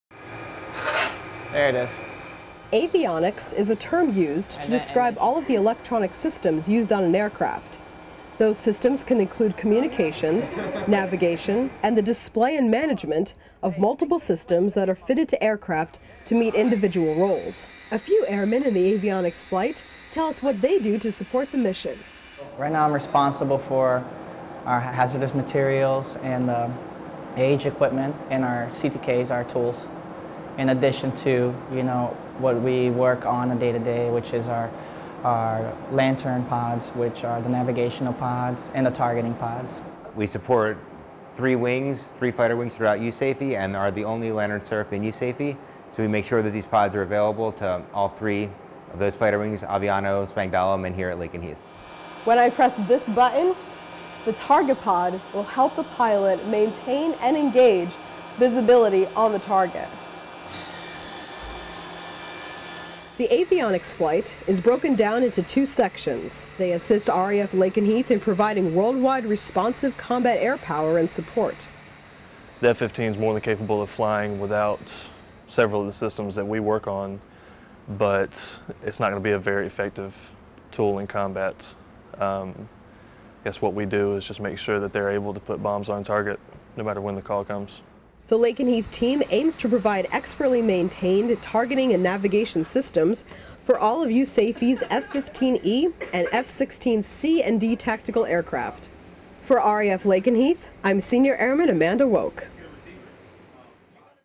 Airmen Maintaining F-15 Fighter Jets' Avionics
At RAF Lakenheath, pilots are often celebrated for their skills and bravery and jets are looked upon with awe and amazement. But behind every jet, there's airmen working to keep the bombs on target. Senior Airman Amanda Wowk takes us "behind-the-scenes" to the avionics flight where the airmen are helping to guide missiles to their proper destinations. Video by Senior Airman Amanda Wowk | 48th Fighter Wing Public Affairs | Date: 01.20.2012

AiirSource℠ covers events and missions from the Army, Navy, Marine Corps, Air Force, and Coast Guard.